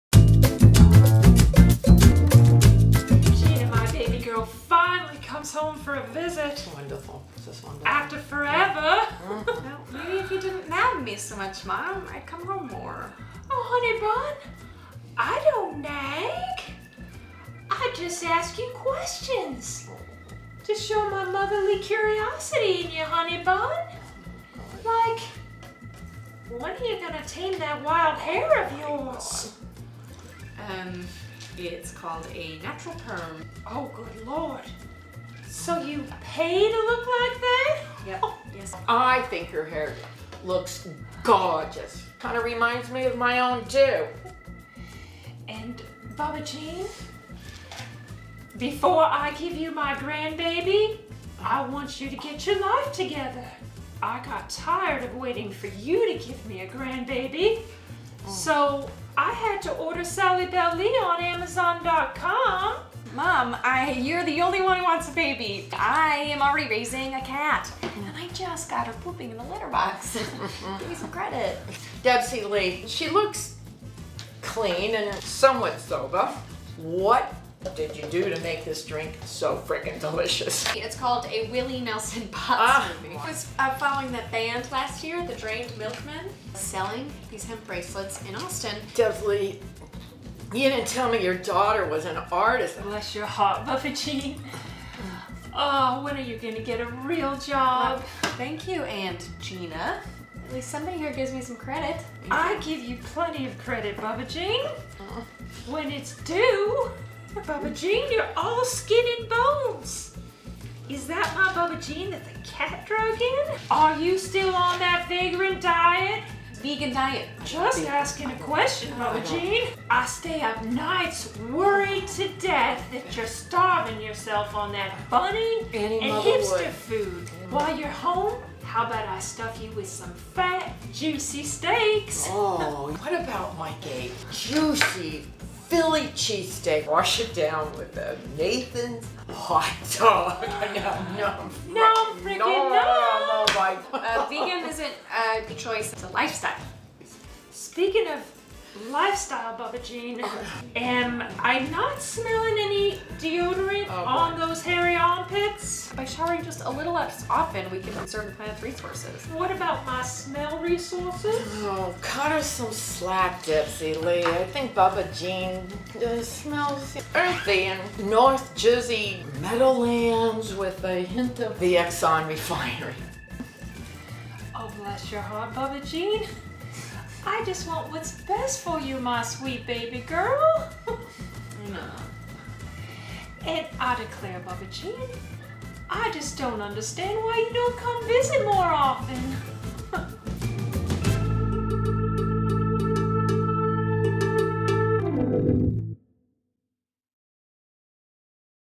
Delusional Housewives of New Jersey - Debsy Lee's Daughter Visits
Debsy Lee's Daughter, Bubba Jean, comes home for a long-awaited visit.